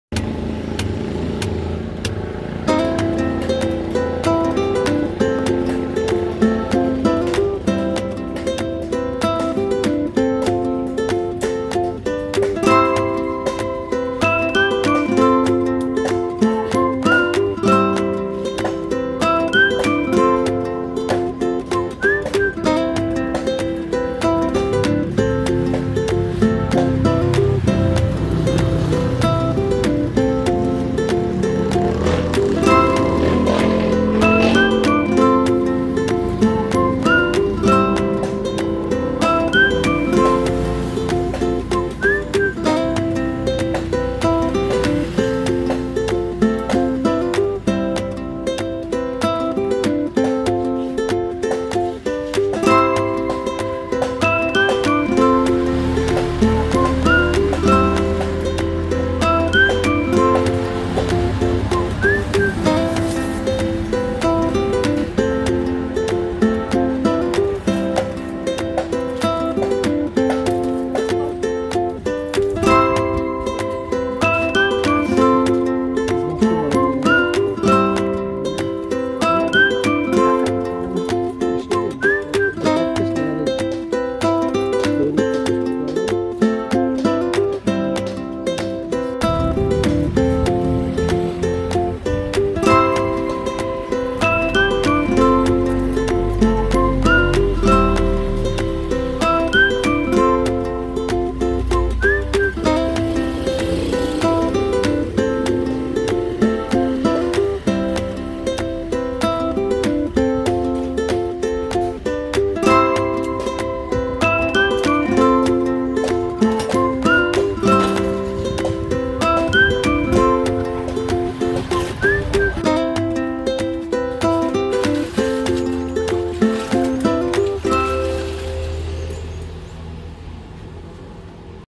Kangkong Tying @mcbalitofarmtv4710
Kangkong(water spinach) tying for display in the store @mcbalitofarmtv4710